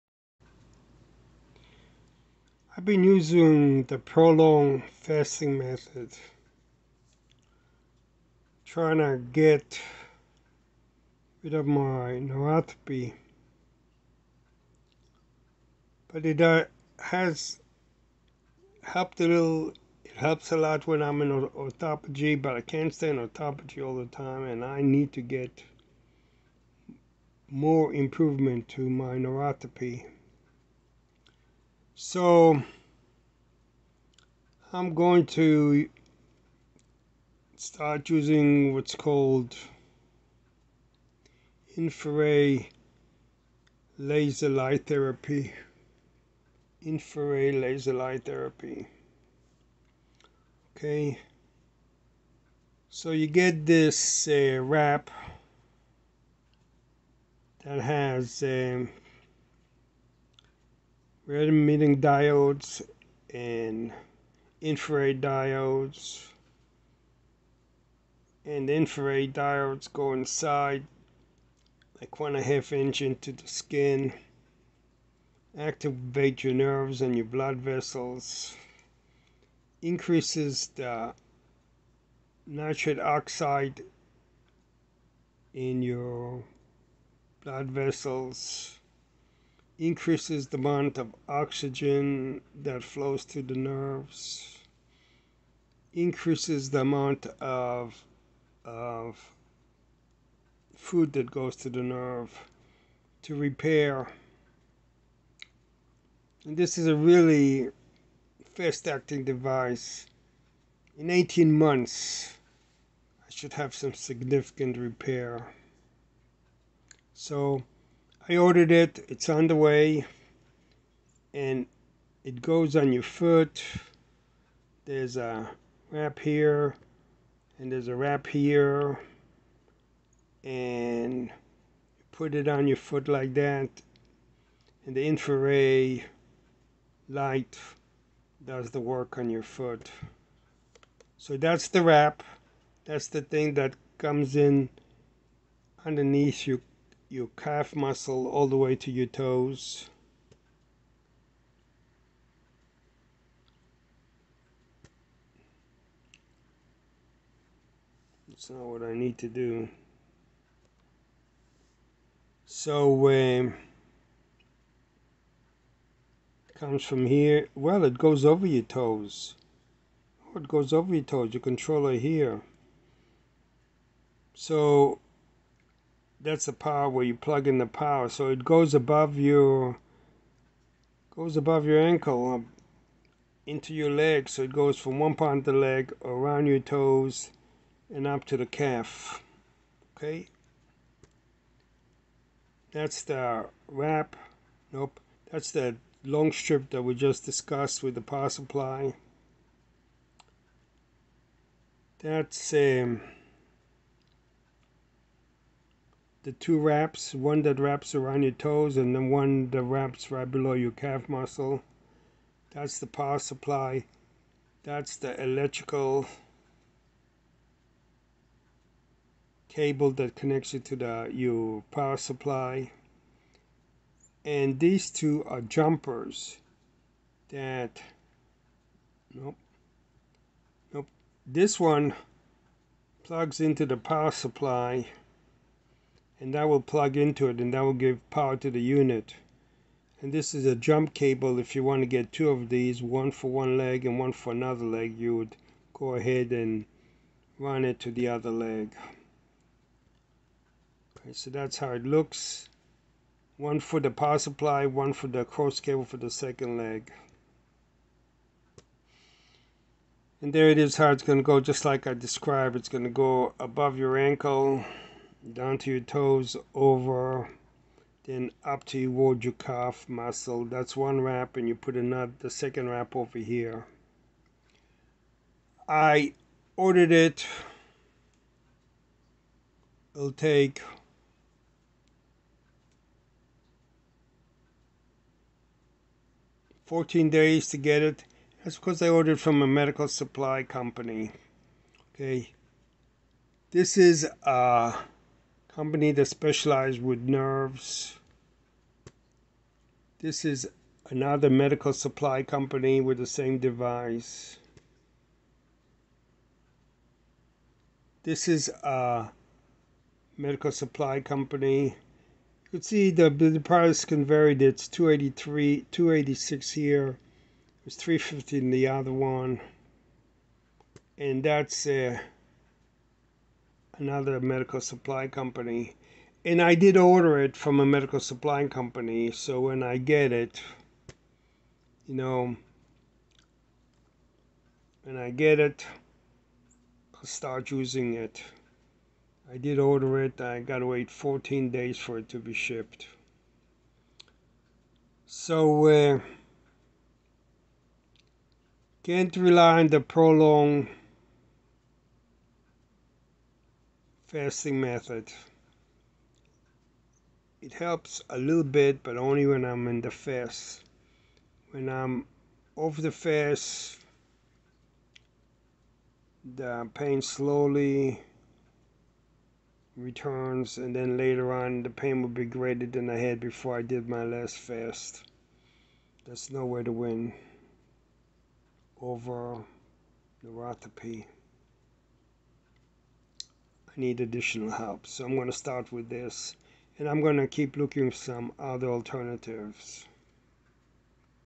INFRARED LIGHT THERAPY AND NEUROPATHY
I have completed ten cycles with the Prolon Fasting protocol.  I am not very happy with my response.  I have to deal directly with the neuropathy.  I was hoping that it was similar to diabetes and I as I went on the Prolon diet it would somehow dissipate, but it is not.

So, now I am beginning to use the infrared light laser diode technology.  It is a famous method, just all over the internet.  I researched a medical grade piece of equipment.  I bought the equipment from a medial supply company.
In fact, to complete the sale, I had to state that I intended to use it for neuropathy.

It is being shipped.  Once it is shipped, I will upload it to the internet.